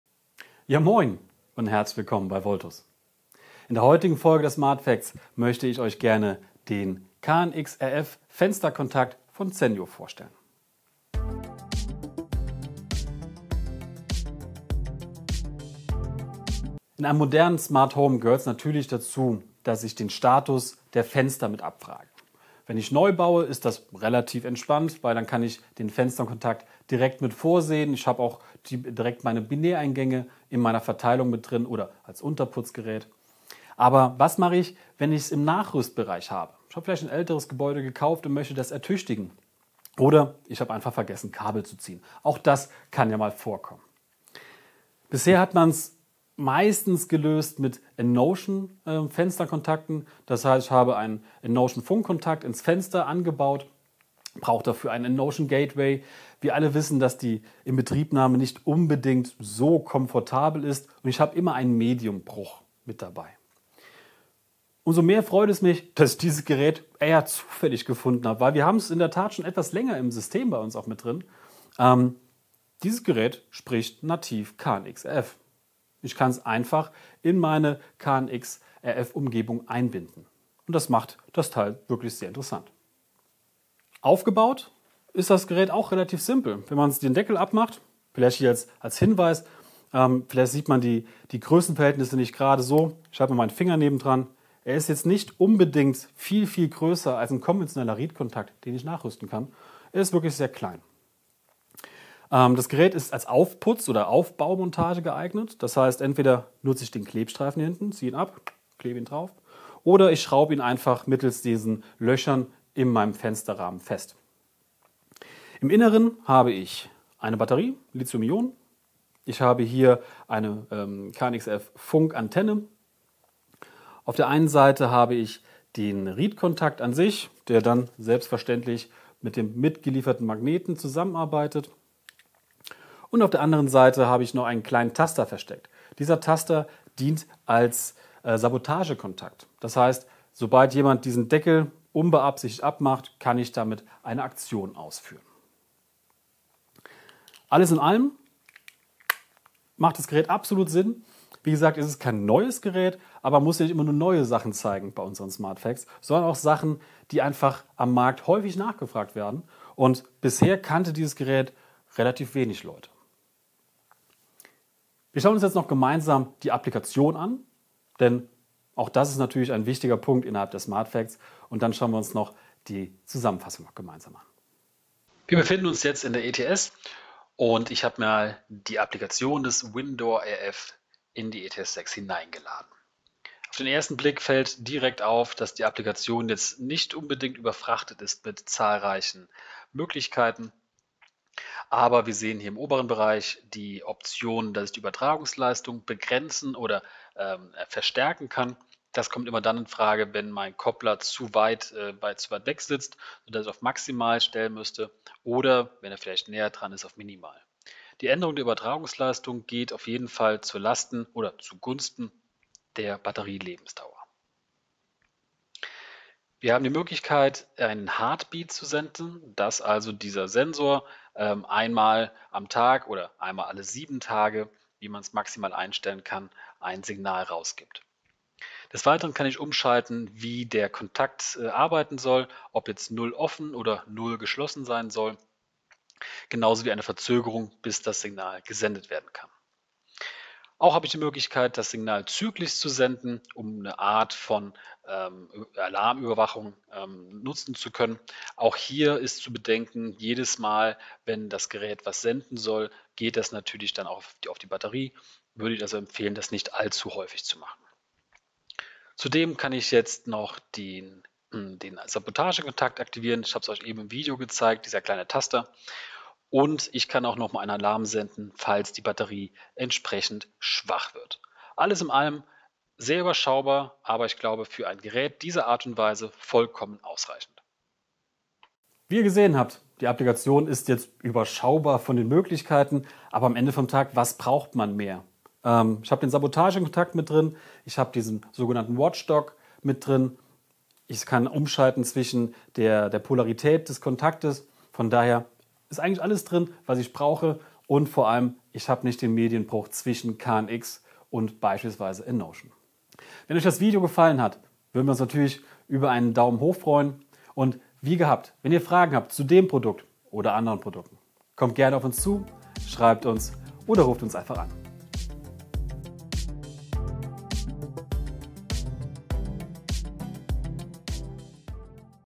Fensterkontakt einfach nachrüsten: Zennio KNX RF
"Ist das Fenster noch offen?" - Der Fensterkontakt von Zennio gibt dir darüber Bescheid. Toll, dass das kleine KNX-Gerät über Funk arbeitet. Auch schön: Du kannst es einfach auf dem Rahmen befestigen. Beides macht das Nachrüsten einfach. Aber schau selbst…

Produktbezeichnung: ZENNIO ZRFWDW KNX-RF Magnetischer Tür- und Fensterkontakt | Standard | weiß
Herstellernummer: ZRFWDW

Kapitel:
00:10: Intro
01:25: Aufbau
02:55: Applikation
04:38: Zusammenfassung
05:14: Outro

Du möchtest nichts mehr verpassen?